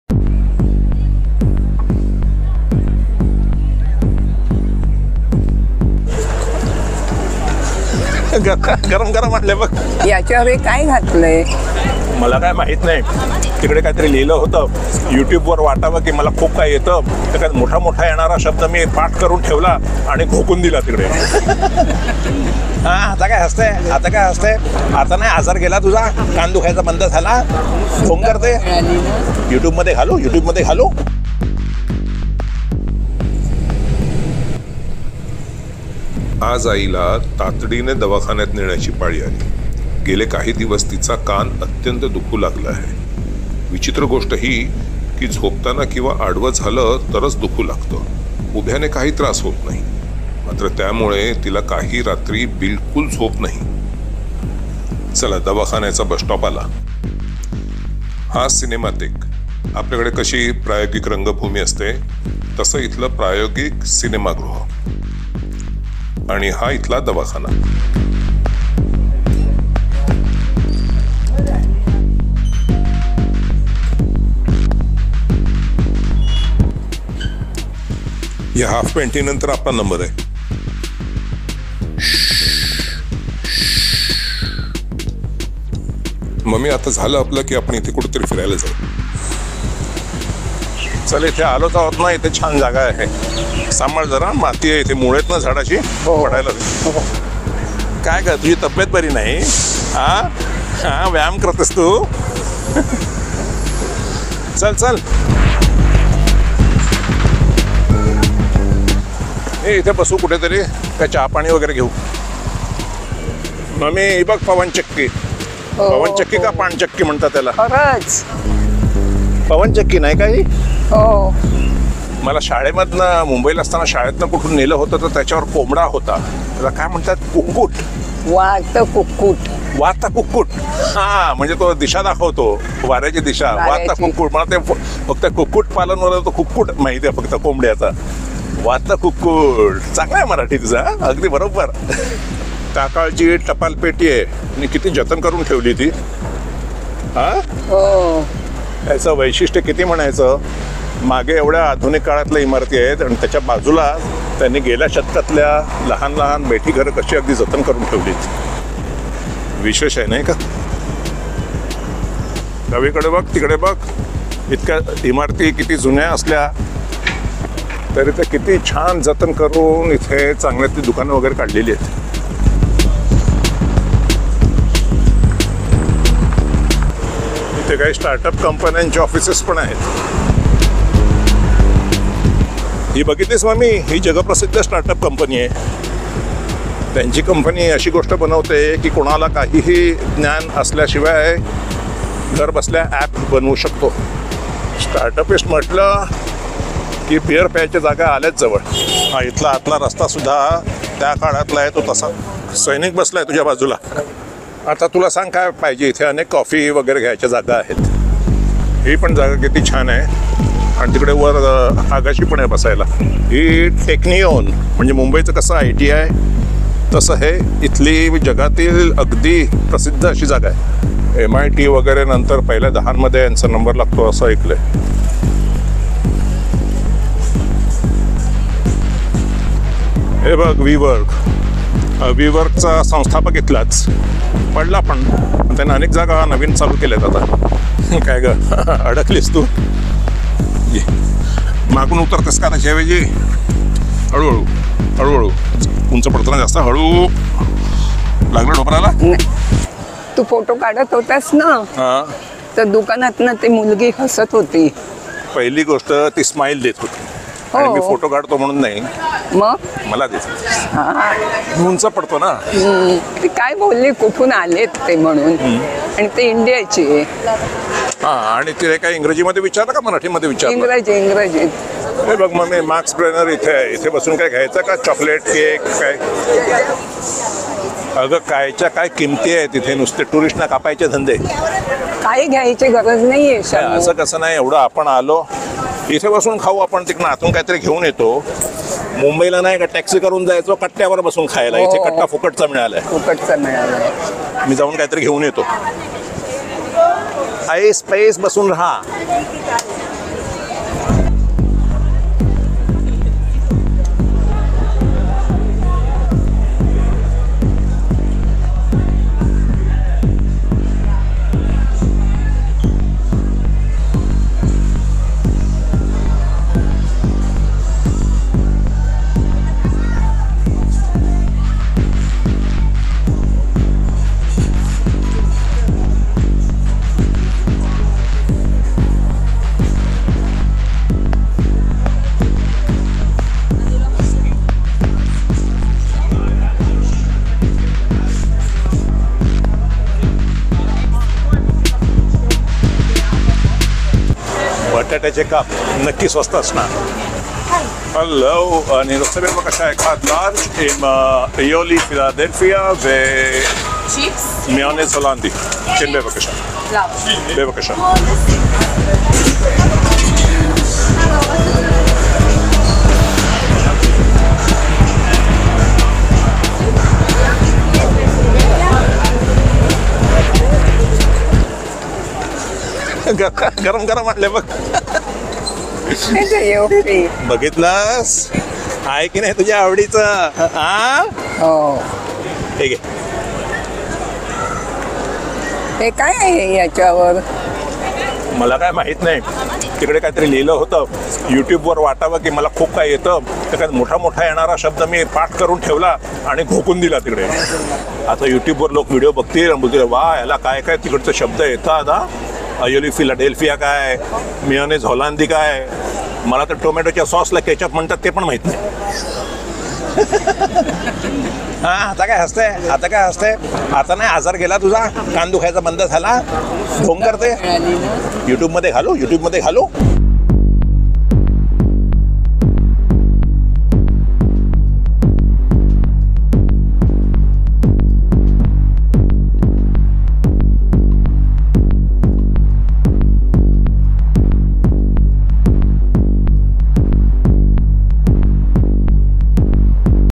Maharashtrian You Tubers Abroad in a Dispensary & Vlog In Marathi.
Marathi Vloggers abroad Maharashtrian mother & son visit a dispensary and later go to a food market. Their Indian lifestyle seems intact in this Marathi vlog though living far away from Maharashtra. Both mother & son don't give up the local food market though they miss the Maharashtrian street food from Mumbai.
आता काय सांगू तुम्हाला, आईचा अचानक कान लागला दुखू. आणि हे दुखणं पण जरा विचित्रच. झोपलो किंवा जरी थोडं आडवं झालं की कान जोरात दुखू लागतो. उभ राहिल की मात्र सर्व दुखणं गायब. मात्र यामुळे रात्रीची झोप तिची बिलकुल उडाली. आज पहाटे तर थकून गेल्यामुळे तिला बिछान्यातून बाहेर पडणे सुद्धा कठीण गेले. मी तातडीने तिला येथील दवाखान्यात न्यायचे ठरवले. बाहेर पडताच तिचा आजार गायब झालेला दिसला. आधीच तिला उभ्याने काही त्रास होत नसेल आणि त्यात बाहेर फिरायची भर अशी दुप्पट गंमत झाली. तर मग काय सांगता, दवाखान्यातून बाहेर पडताच तेथील जवळच अशा करमणुकीच्या ठिकाणी जाऊन फिरलो. खाण्याच्या गोष्टी जरा महाग होत्या मात्र लहान सहान घेऊन मजा मात्र पूर्ण केली. महाराष्ट्रीयन खाद्यपदार्थ इथे मिळत नाहीत याची उणीव फार जाणवते. आता मुंबईला असतो तर बटाटे वडे, कांदे भजी, मिसळपाव, मसाला डोसा आणि काही नाही ते खाल्ले असते. मात्र आईच्या चेहऱ्यावरील आनंद पाहून ती सर्व उणीव भरून निघाली.
00:00 Introduction
00:11 Missing Maharashtrian street food
00:37 Maharashtrian mother taken to the dispensary
01:06 Dispensary
01:37 Marathi Mom in playground
01:54 Windmill
02:57 Israel Startup offices
03:35 Technion
03:52 Wework
04:03 Helping mother
04:32 Max Brenner Cafe
05:09 Street food market
05:23 Moroccan Coffee
05:34 Arabic sweets
05:58 Chips with Ioli philadelphia and Dutch Mayonise
06:10 Deep fried chips
06:21 Maharashtrian mother and son eating togeather
06:32 Missing general knowledge of a Maharashtrian